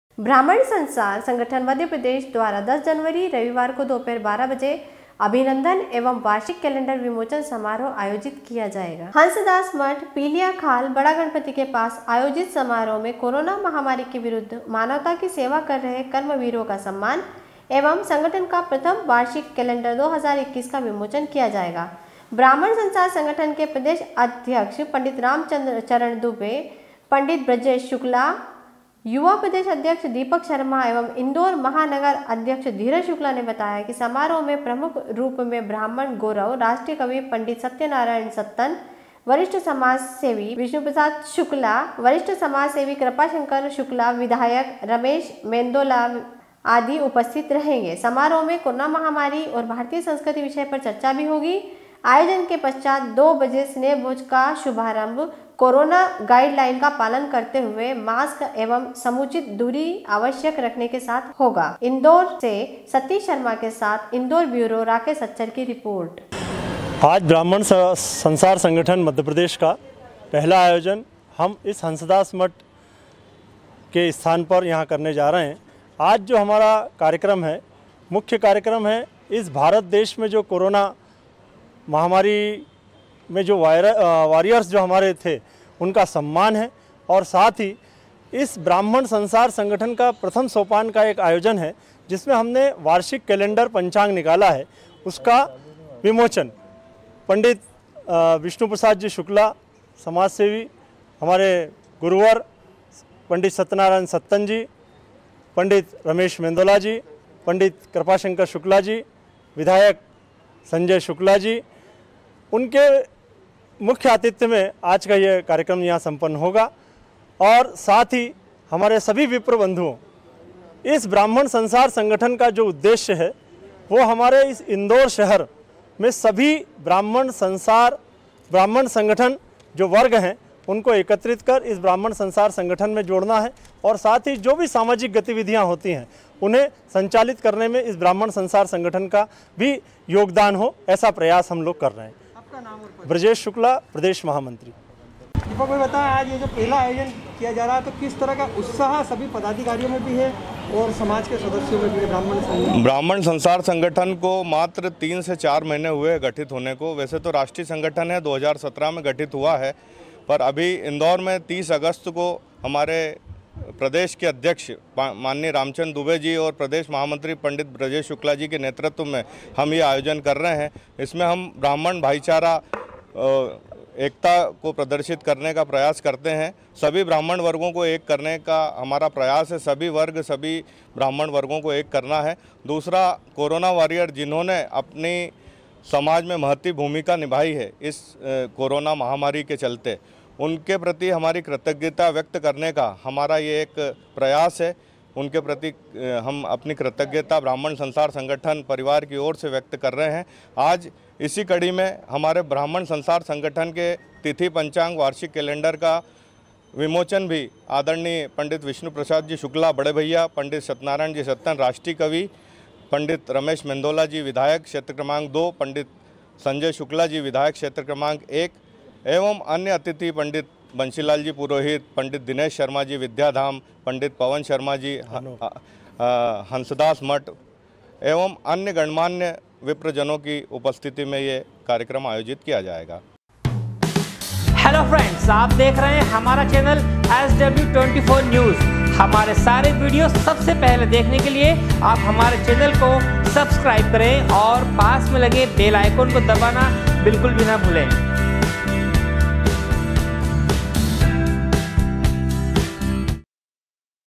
Indore- दोपहर 12 बजे अभिनंदन एवं वार्षिक कैलेंडर विमोचन समारोह आयोजित किया जाएगा।
ब्राह्मण संसार संगठन, मध्य प्रदेश द्वारा 10 जनवरी रविवार को दोपहर 12 बजे अभिनंदन एवं वार्षिक कैलेंडर विमोचन समारोह आयोजित किया जाएगा। इंदौर ब्यूरो राकेश सच्चर की रिपोर्ट